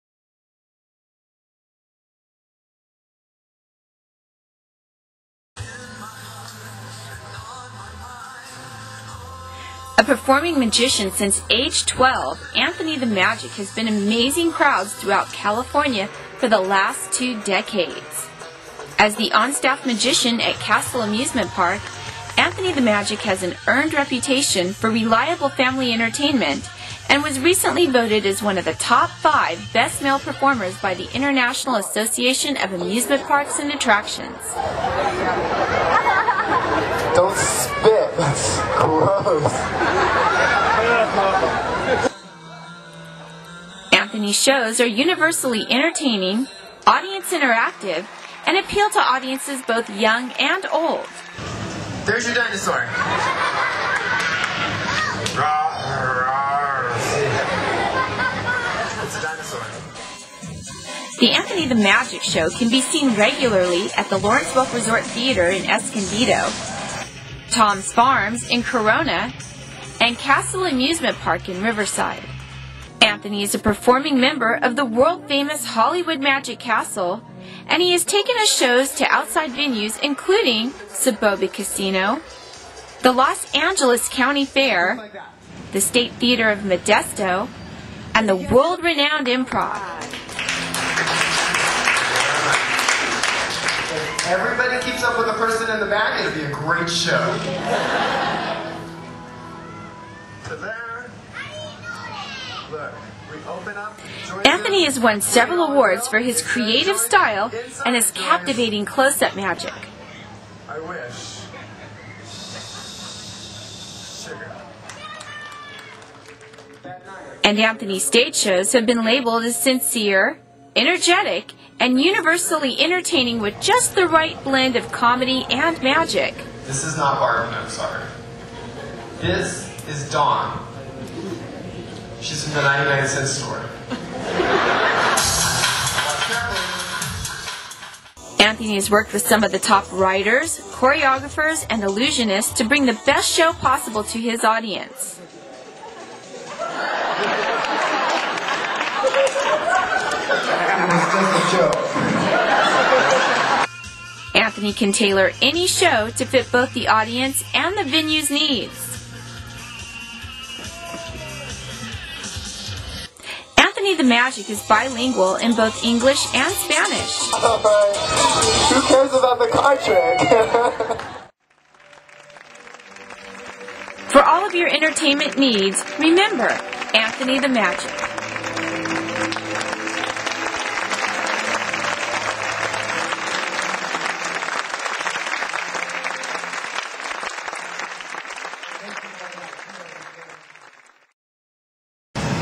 Anthony the Magic 2007 Demo Tape
Anthony the Magic is one of the premier magicians in Southern California.  Voted as one of the "Top 5 Best Male Performers" by the International Association of Amusement Parks, Anthony has taken his show to the L. A. County Fair, the State Theatre of Modesto, Soboba Casino, the Improv, and the Lawrence Welk Resort Theatre.